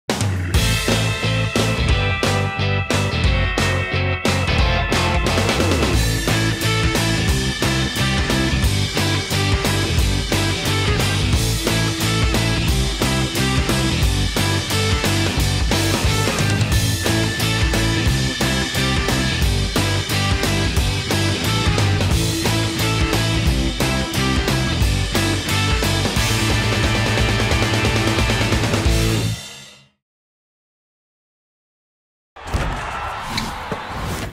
Juan NÚÑEZ, Candidato Mejor Joven | Liga Endesa 2021-22
El segundo más joven del listado ya ha participado 13 encuentros en todo un Real Madrid sin cumplir la mayoría de edad, regalando algún destello que invita a la ilusión con él. Definitivamente, Juan Nuñez es uno más de la plantilla.
¡Entra en nuestra app y vota por el Mejor Joven de la Liga Endesa!